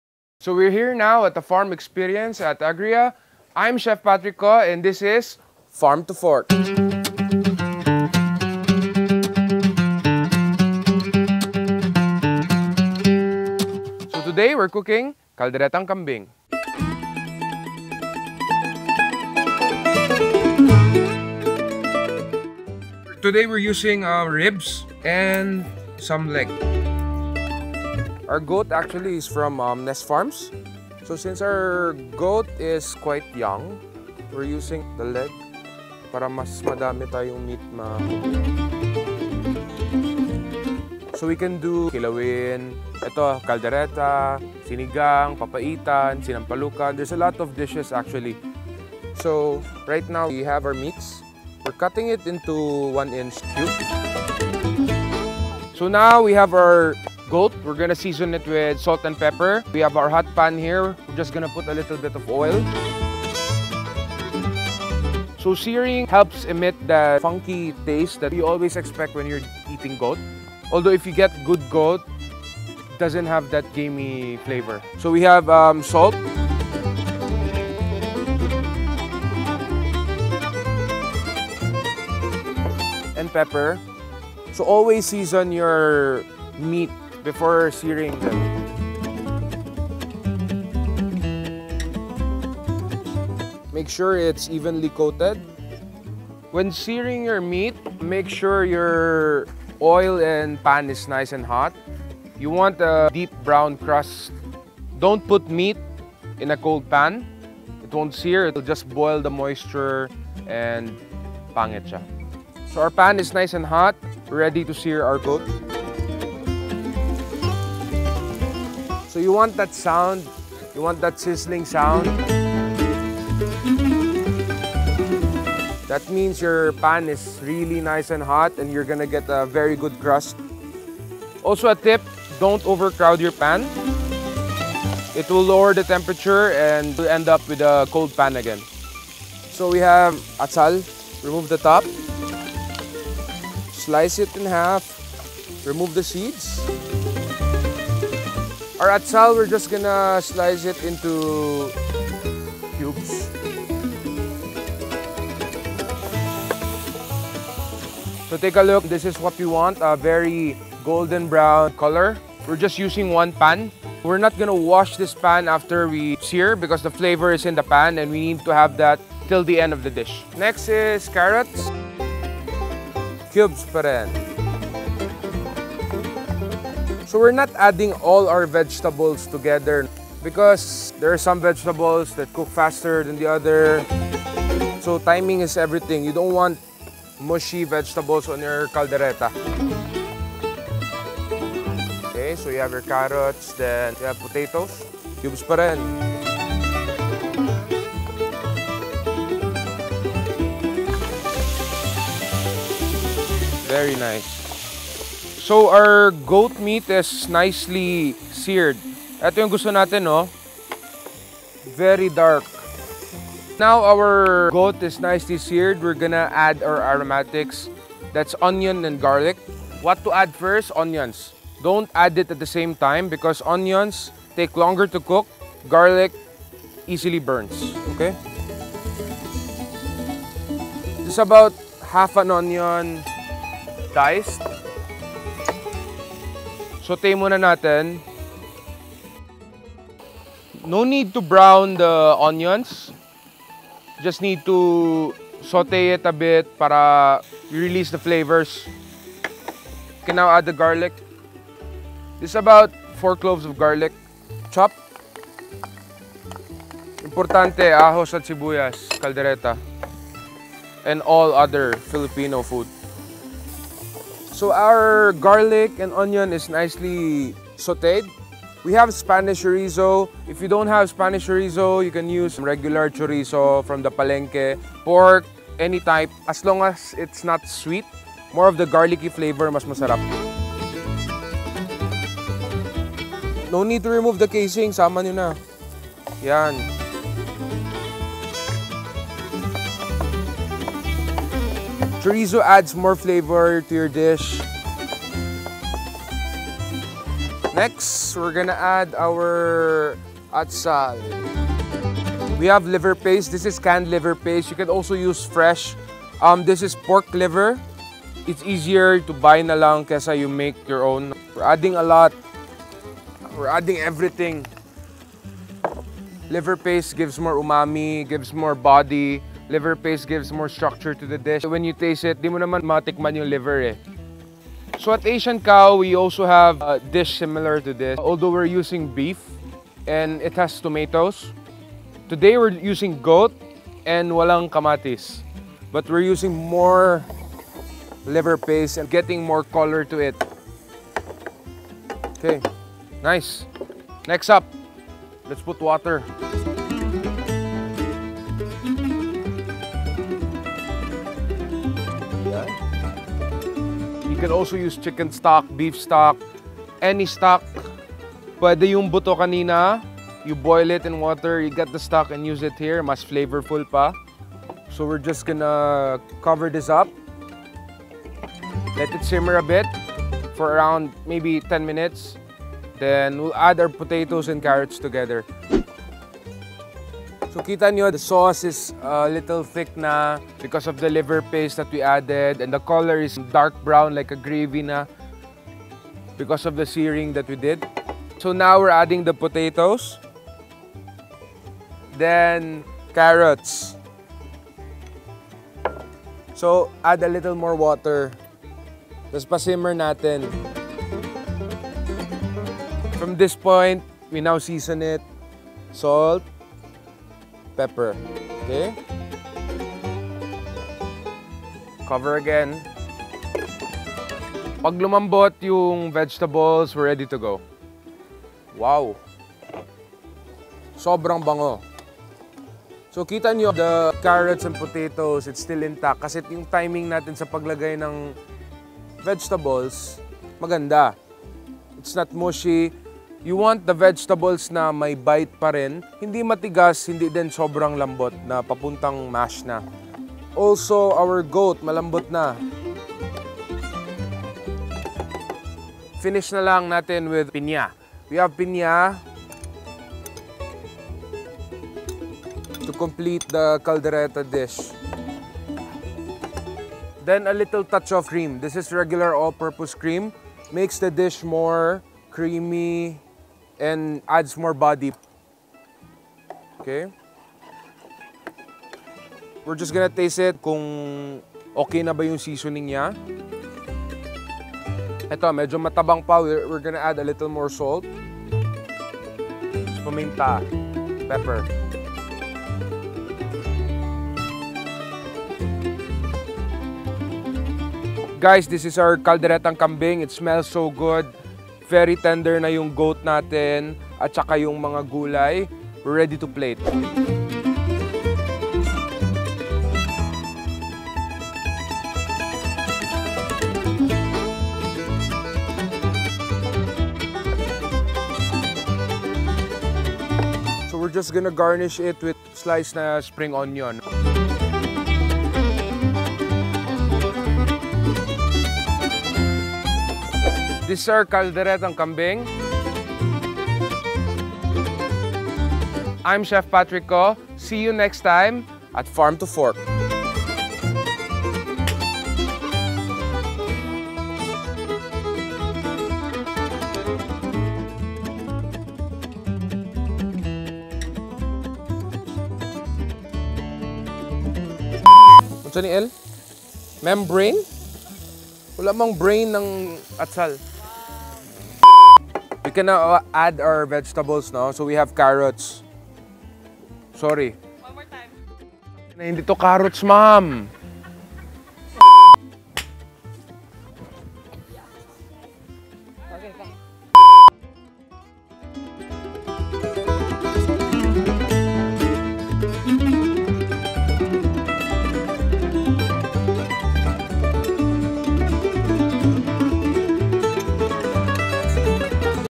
Farm-to-Fork With Chef Patrick Co - Episode 4
Kalderetang Kambing anyone? Learn how to cook this popular Filipino favorite from no less than Davao’s very own Chef Patrick Co of The Fat Cow!

See more of Farm to Fork episodes at Damosa Land 360!